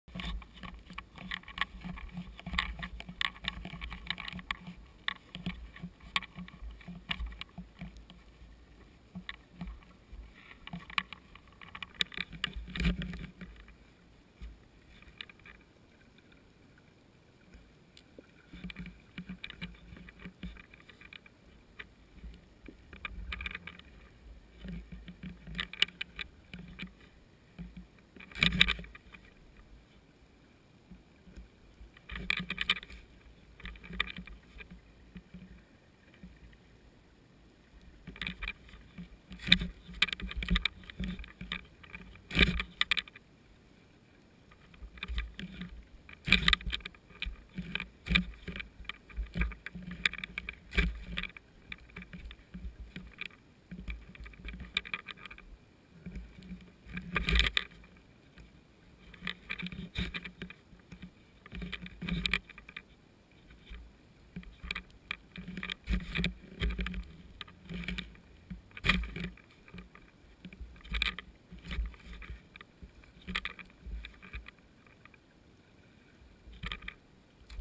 170 feet deep Gulf of Mexico mystery fish
Wondering, what type of fish is it in the hole in the back? This is the best footage, shortly after we drifted away from the spot. Warsaw grouper? Goliath Grouper?